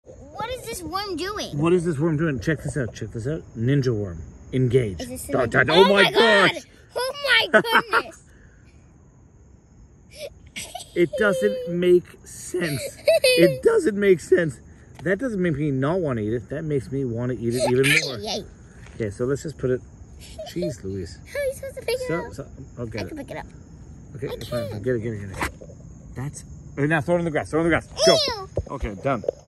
Florida Worms are CRAZY!
Seriously, this is incredible. To think that they have evolved to react like this to better fend off predators.￼